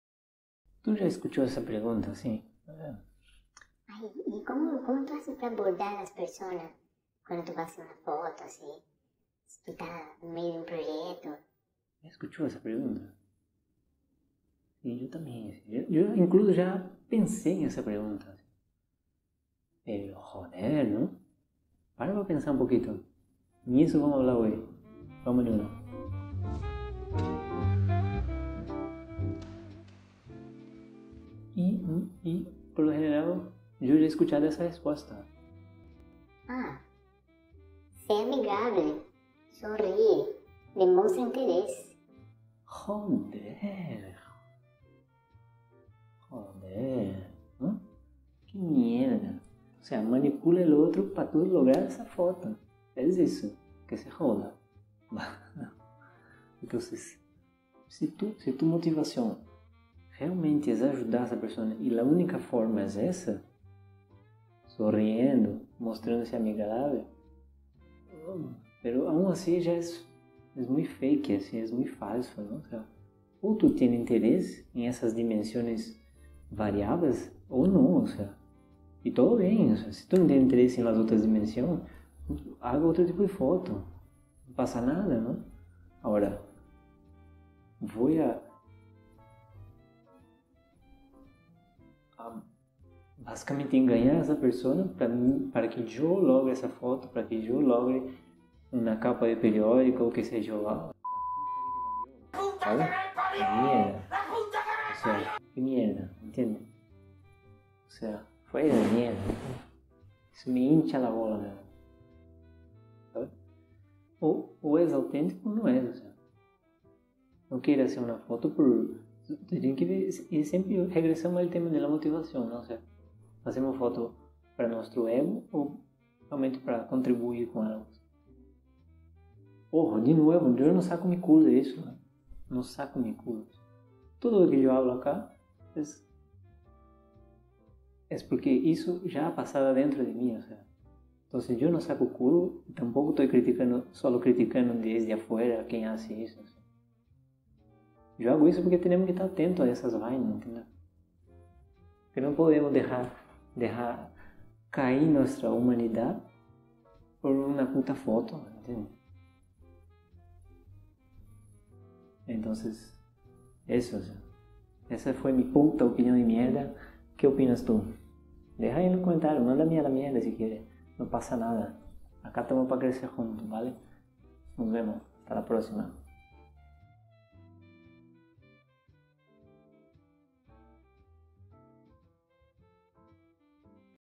Abordar al Sujeto a ser Fotografiado - E5
La duda sobre como abordar al sujeto o grupo a ser fotografiado. Reflexiono sobre esas dudas, sobre la autenticidad y la motivación. Me he exaltado un poco, pero, como siempre, solo mi perspectiva limitada.

Cosas que comparto, cosas que respecto pero no comparto y lo que más se me venga...

Estoy empezando una nueva serie, un nuevo espacio dentro de ese canal, llamado DARKROOM EN USO, un espacio para pensar, reflexionar, filosofar "negativos temáticos" relacionados con el arte de la fotografía.

Acá no se pretende llegar a ninguna verdad o resolución, quizás más cuestionamientos.
Un espacio para compartir perspectivas y etc.